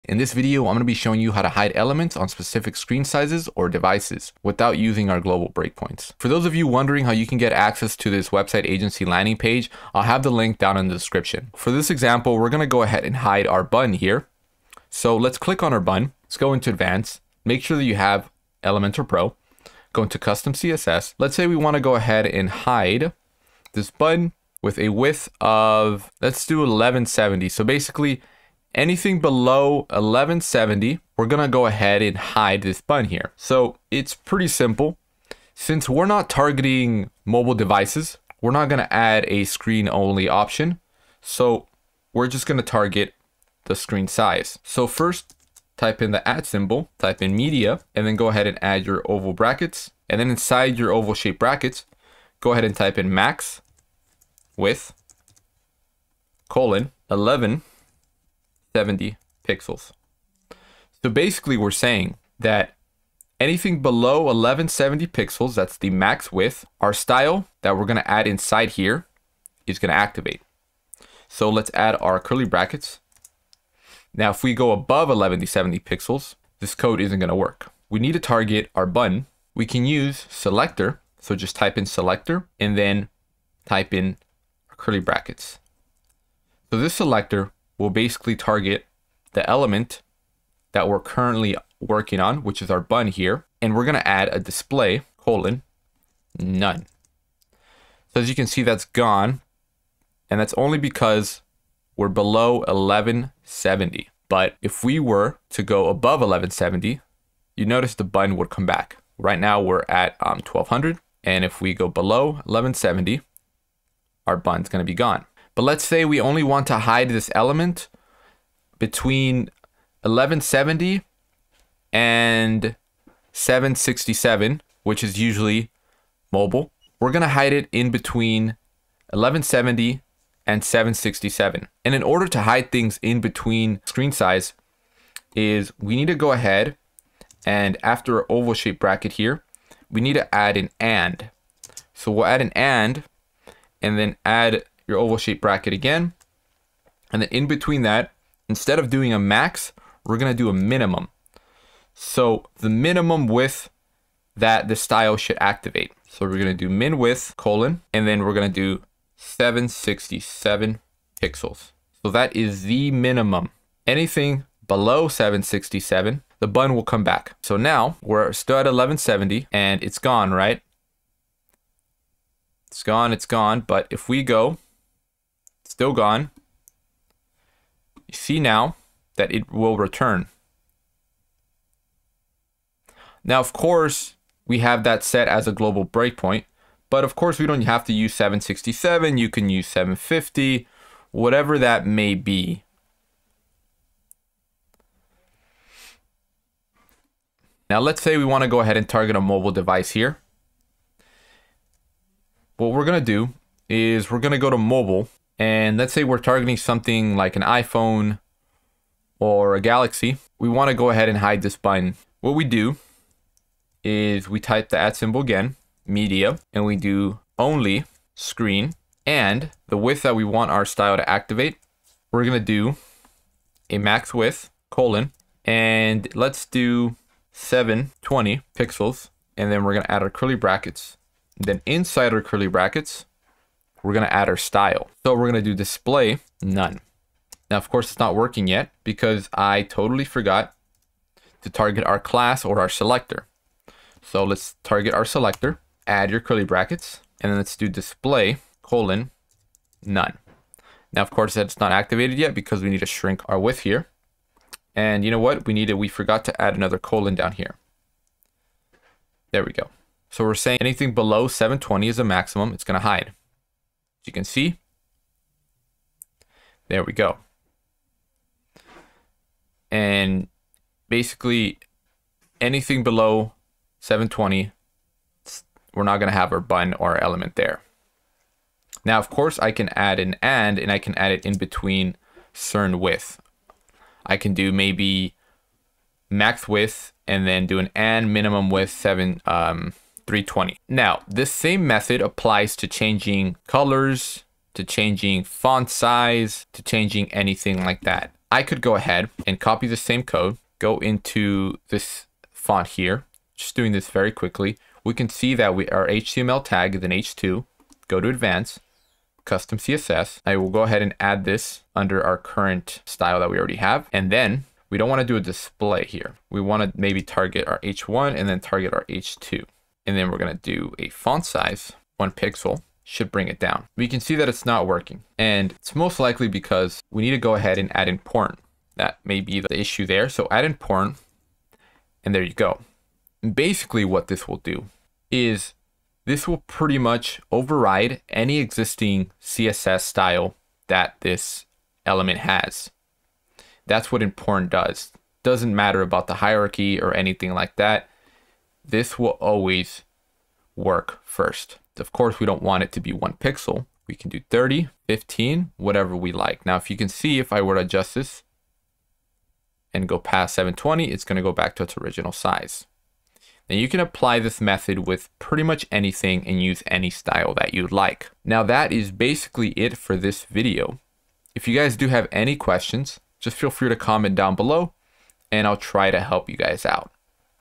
Hide elements on any screen size or mobile device In Elementor Pro
Learn how to hide elements on any screen size in Elementor pro using custom breakpoint media queries.

CSS:

@media (max-width:650px) and (min-width:450px){
selector{
display:none;

selector{
display:none;